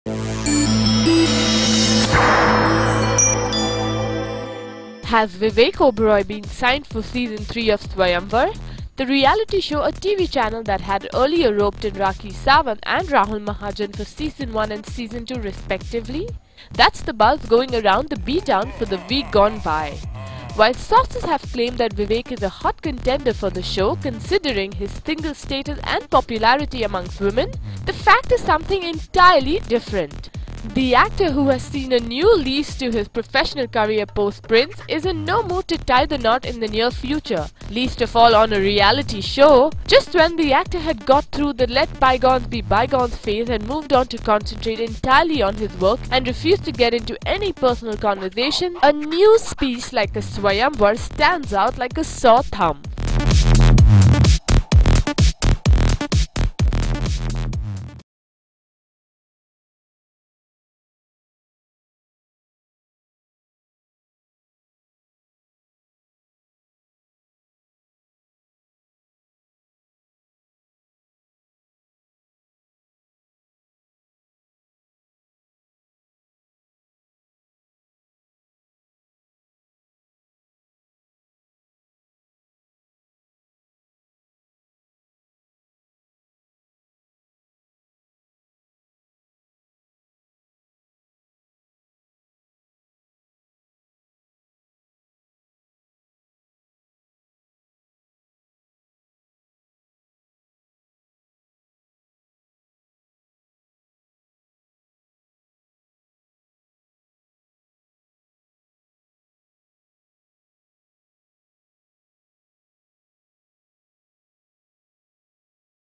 Vivek Oberoi in 'Swayamvar-3'
Has Vivek Oberoi been signed for Season 3 of 'Swayamvar', the reality show at a TV channel that had earlier roped in Rakhi Sawant and Rahul Mahajan for Season 1 and Season 2 respectively?